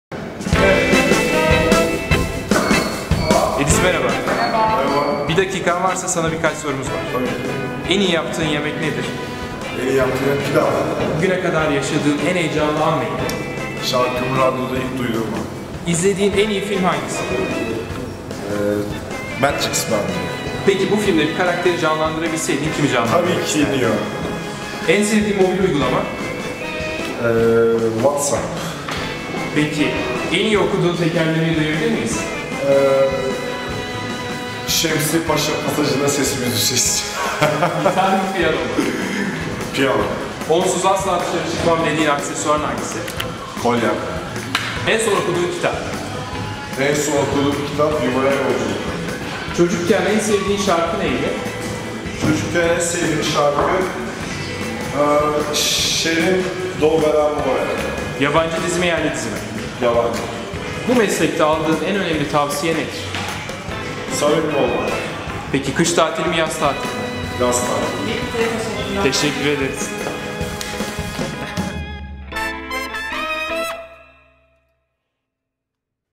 1 Dakikada Edis'i Tanıyın!
GQ Türkiye, 2015’te başarıları ve tarzlarıyla dikkat çeken erkekleri ve yılın kadınını seçti. Edis, GQ Türkiye tarafından “Yılın Müzisyeni” seçildi. Bunun için yapılan fotoğraf çekiminde Edis'i 1 dakikada soru yağmura tutan GQ ekibinin hazırladığı o video geliyor!